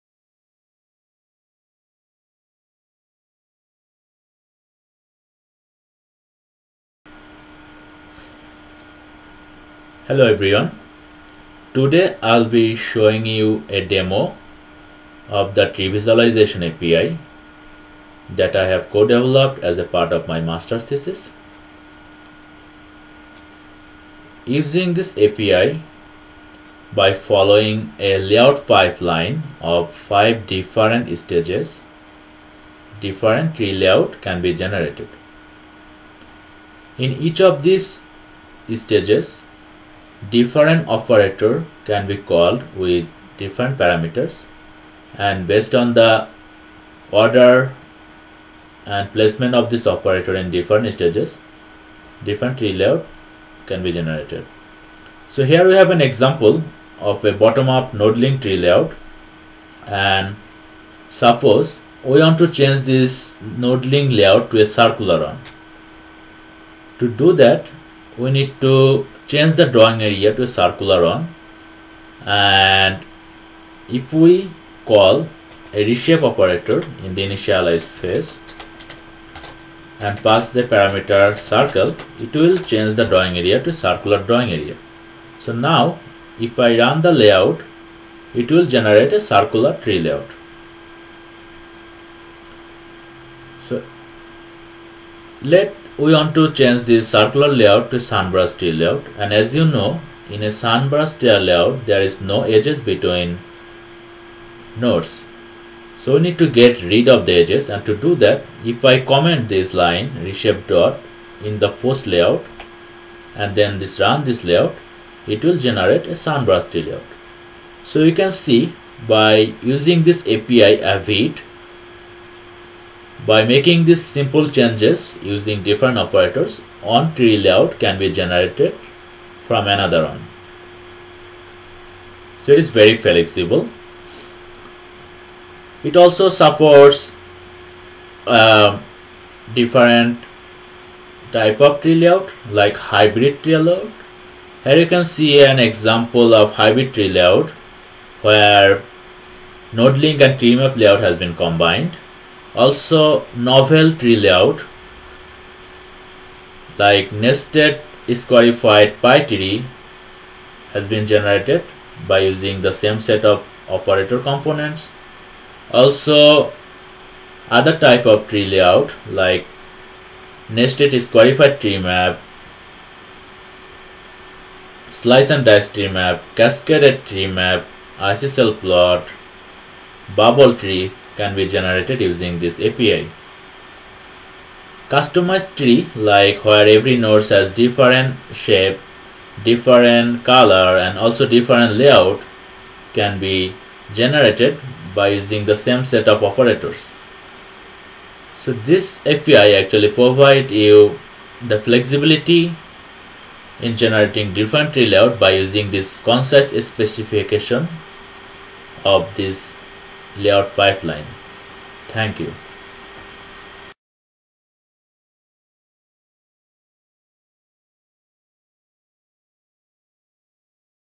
AVIT: An API for Visualizing and Interacting with Trees
AVIT: API for Visualizing  and Interacting with Trees
By, Hans-Jörg Schulz, Zabedul  Akbar, Frank Maurer

Technology Used: JavaScript, JQuery, HTML 5, SVG, CSS.